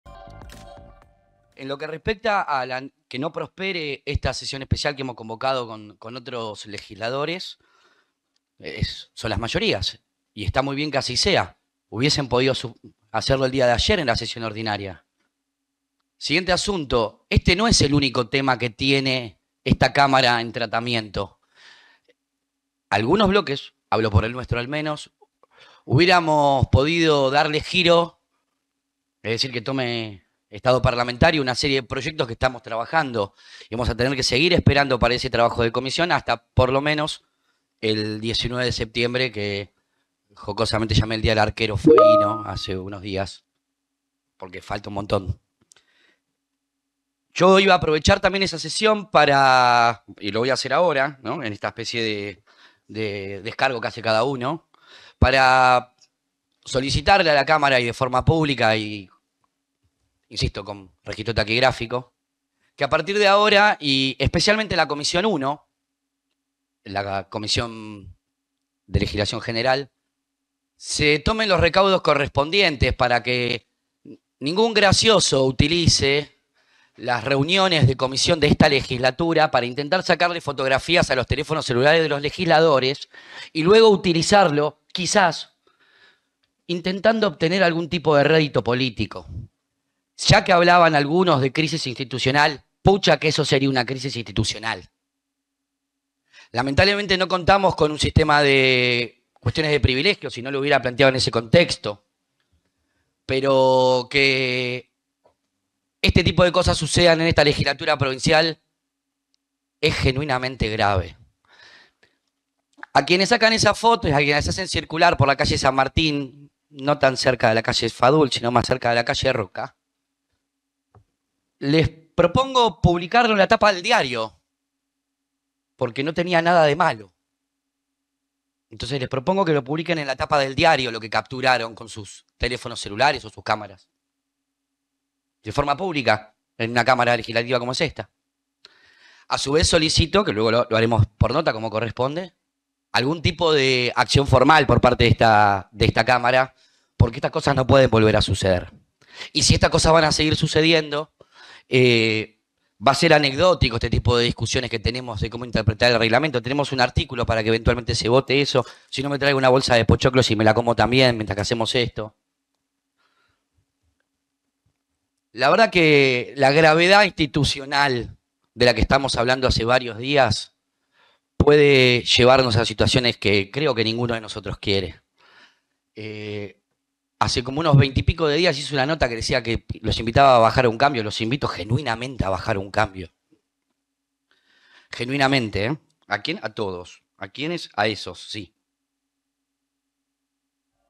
SESIÓN ESPECIAL CONVOCADA POR SIETE LEGISLADORES OPOSITORES
Agustín Coto, el legislador de la Libertad Avanza, se quejó de unas fotos que tomaron a su celular y que quisieron utilizar políticamente apuntando a sectores del  gobierno provincial. Y pidió “bajar un cambio”.